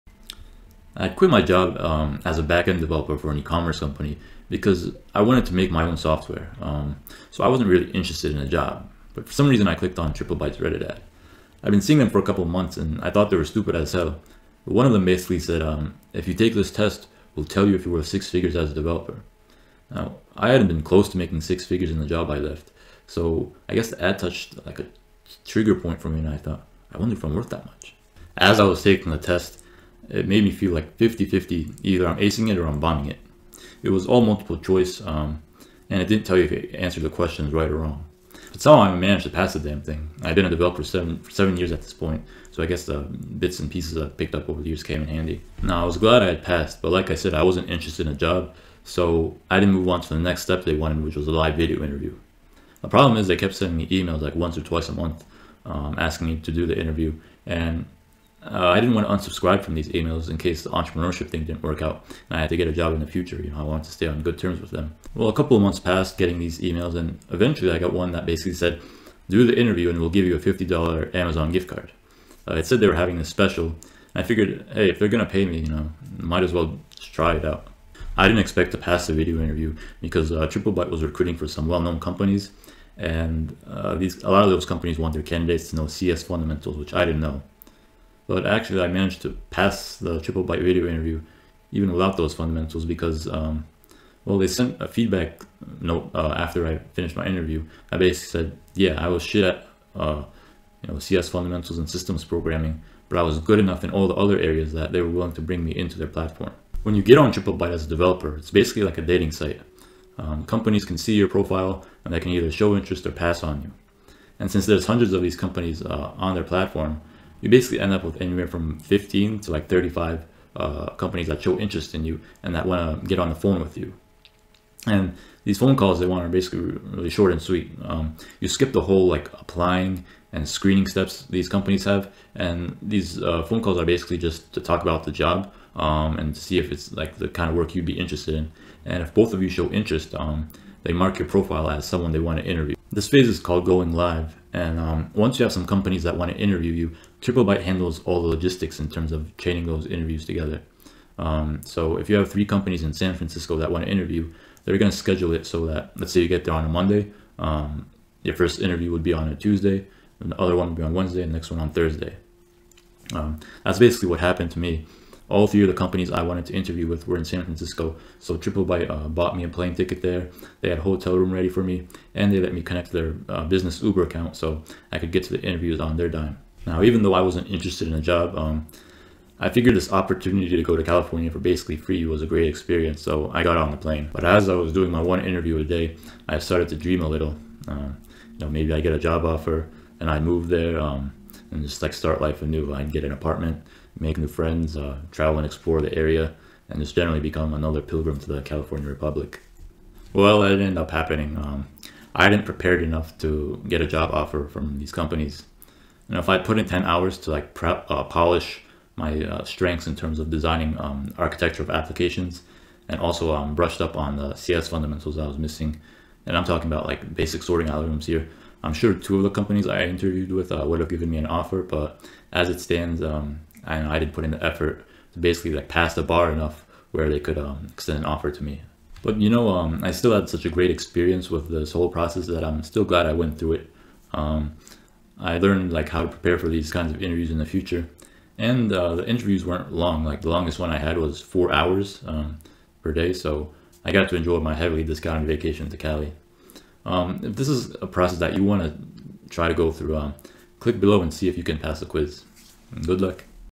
Triplebyte Quiz Review for Developers and Coders Looking to Find a Better Job
A 15 minute coding quiz can save you 20 hours of job searching.
You could turn 15 minutes into $149,000.
The fact is more than half the developers who pass through this process receive a job offer for $149,000 *cash* -- before benefits and equity.
It doesn't matter if you have a job or no job.
It doesn't matter if you live on a creek in Idaho or San Francisco California.
Your resume is irrelevant. The only question is, are you a good enough programmer?
To answer that, you have to take the quiz and pass.